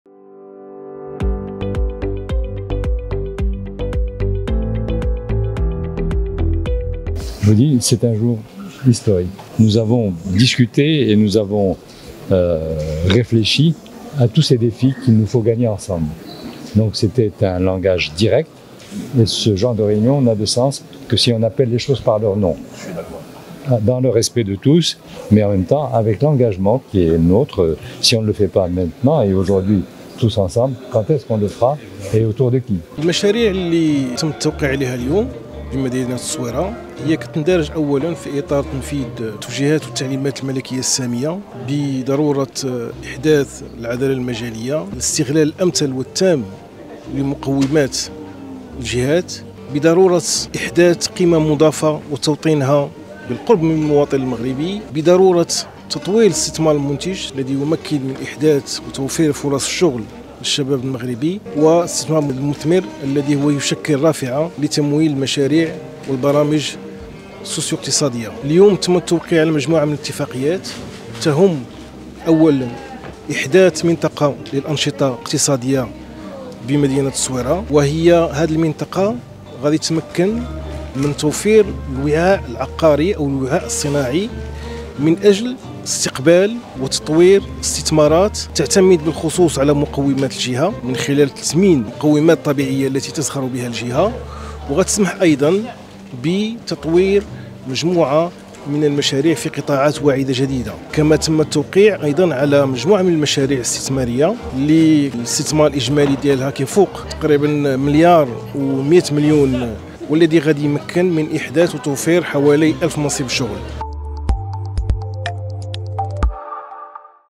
التوقيع على 6 اتفاقيات مهمة ضمنها اتفاقية لإحداث منطقة للأنشطة الاقتصادية بالصويرة
على هامش انعقاد أشغالِ "يوم المستثمر للصويرة" ، قام السيد توفيق مشرف، الكاتب العام لوزارة الصناعة والتجارة، بالتوقيع على 6 اتفاقيات مهمة ضمنها اتفاقية لإحداث منطقة للأنشطة الاقتصادية بالصويرة من أجل استقبال وتطوير الاستثمارات التي تعتمد على مقومات جهة مراكش آسفي.

وشملت الاتفاقيات التي تم التوقيع عليها تطوير مجالات صناعية واستثمارية تهم مجال صناعة الأغذية والسيارات الكهربائية والمنتجات الطبية الصيدلانية وتجميع معدات المختبرات بإقليم الصويرة، بمبلغ مالي إجمالي يناهز مليار و135 مليون درهم، لتوفير 888 فرصة شغل على مستوى الإقليم.

هذه المشاريع تندرج في إطار تنفيذ التعليمات الملكية السامية القاضية بضرورة إحداث العدالة المجالية والاستغلال الأمثل والتام لكل مقومات الجهات وبضرورة إحداث قيم مضافة وتوطينها بالقرب من المواطن المغربي، إضافة إلى تعزيز تطوير الاستثمار المغربي المنتج الذي يمكن من إحداث وتوفير فرص شغل للشباب المغربي، كما تنخرط هذه المشاريع في إطار الارتقاء النوعي الذي تخرط فيه الصناعة المغربية.